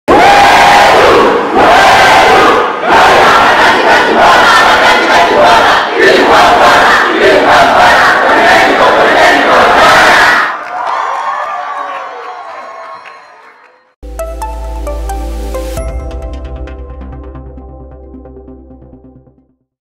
Huélum ESCA Santo Tomás
La comunidad politécnica de la ESCA Santo Tomas se unió en un Huélum masivo para celebrar 178 años de su creación.